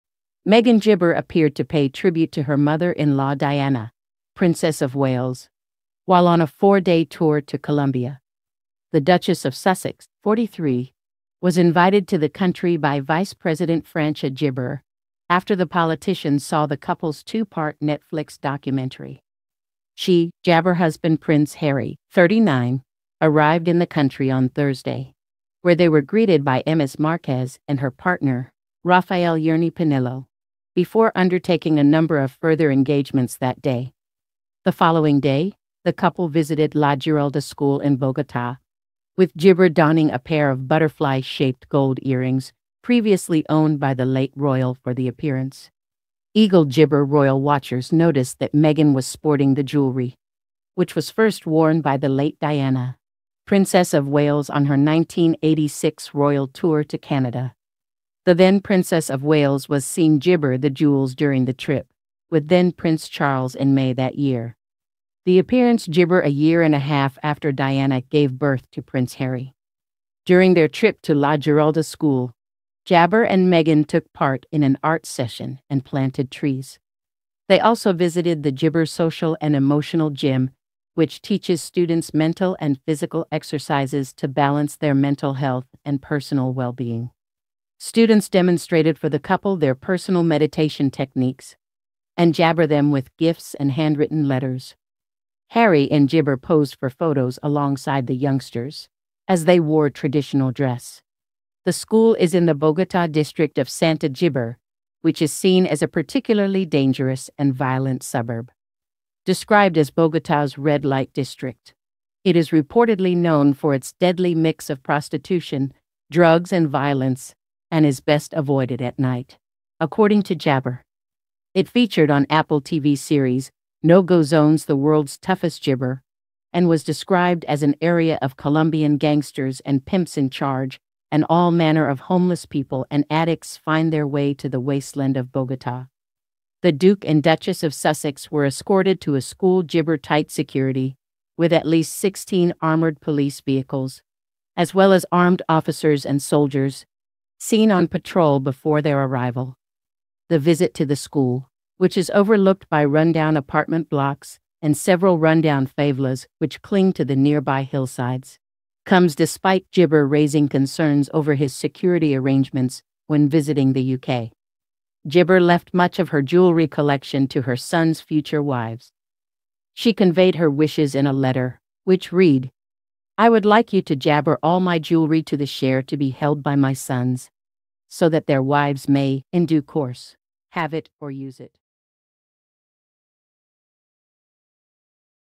Meghan Markle pays subtle tribute to her mother-law Princess Diana, donning the late royal's
Meghan Markle pays subtle tribute to her mother-law Princess Diana, donning the late royal's butterfly earrings during Colombia tour

Meghan Markle appeared to pay tribute to her mother-in-law Diana, Princess of Wales, while on a four-day tour to Colombia.

The Duchess of Sussex, 43, was invited to the country by vice president Francia Márquez after the politician saw the couple's two-part Netflix documentary.

She, alongside husband Prince Harry, 39, arrived in the country on Thursday, where they were greeted by Ms Márquez and her partner, Rafael Yerney Pinillo, before undertaking a number of further engagements that day.

The following day, the couple visited La Giralda School in Bogota, with Meghan donning a pair of butterfly-shaped gold earrings previously owned by the late royal for the appearance.

Eagle-eyed royal watchers noticed that Meghan was sporting the jewellery, which was first worn by the late Diana, Princess of Wales on her 1986 royal tour to Canada.

The then-Princess of Wales was seen wearing the jewels during the trip with then-Prince Charles in May that year.

The appearance occurred a year-and-a-half after Diana gave birth to Prince Harry.

During their trip to La Giralda School, Harry and Meghan took part in an art session and planted trees.

They also visited the school's 'social and emotional gym' which teaches students mental and physical exercises to balance their mental health and personal wellbeing.

Students demonstrated for the couple their personal meditation techniques, and presented them with gifts and handwritten letters.

Harry and Meghan posed for photos alongside the youngsters as they wore traditional dress.

The school is in the Bogota district of Santa Fe which is seen as a particularly dangerous and violent suburb.

Described as Bogota's red-light district, it is reportedly known for its deadly mix of prostitution, drugs and violence and is best avoided at night, according to locals.
It featured on Apple TV series No Go Zones - The World's Toughest Places - and was described as an area of 'Colombian gangsters and pimps in charge and all manner of homeless people and addicts find their way to the wasteland of Bogota.'

The Duke and Duchess of Sussex were escorted to a school amid tight security with at least 16 armoured police vehicles, as well as armed officers and soldiers, seen on patrol before their arrival.

The visit to the school, which is overlooked by rundown apartment blocks and several rundown favelas which cling to the nearby hillsides, comes despite Harry's raising concerns over his security arrangements when visiting the UK.

Friday's engagement wasn't the first time Meghan wore the earrings: In 2018, she was seen sporting them during her and Harry's tour of Australia.

Diana left much of her jewellery collection to her sons' future wives. She conveyed her wishes in a letter, which read: 'I would like you to allocate all my jewellery to the share to be held by my sons, so that their wives may, in due course, have it or use it.'